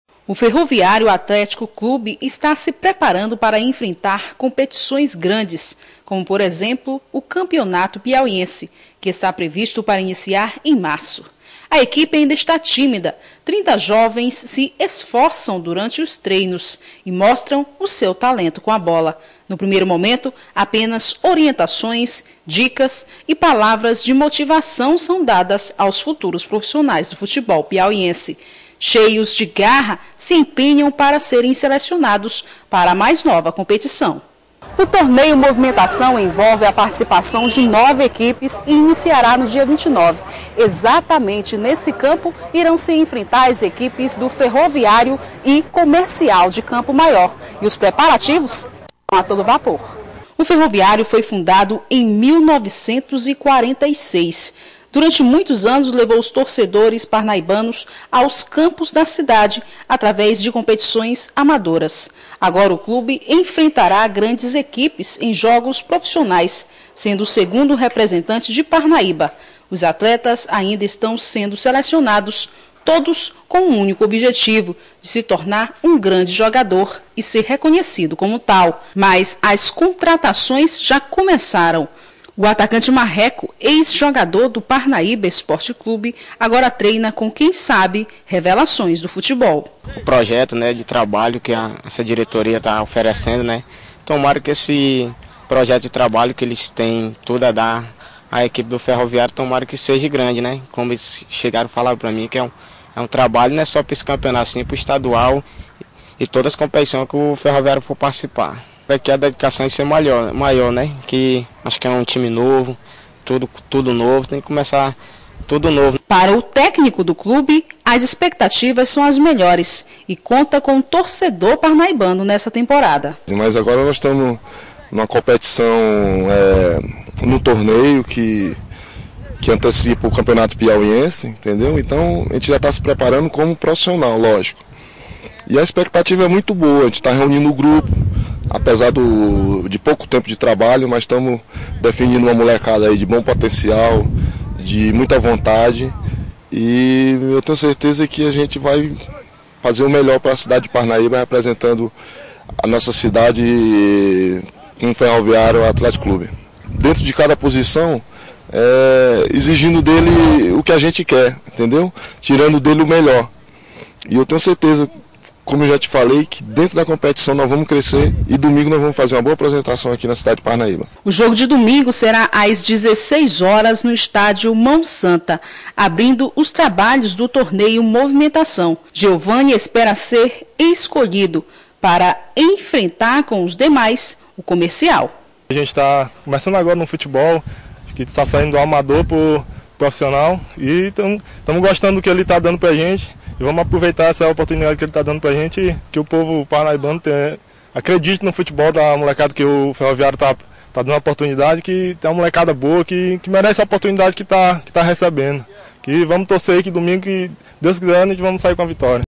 Ferroviário tem semana de preparação para enfrentar o Comercial de CM.flv
Ferroviário tem semana de preparação para enfrentar o Comercial de CM